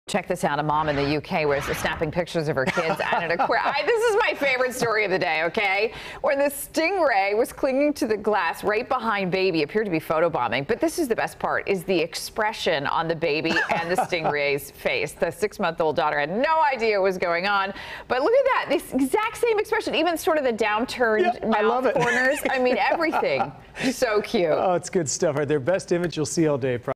Stingray photobombs baby at aquarium
A photo has gone viral of a stingray photobombing a baby while at an U.K. aquarium.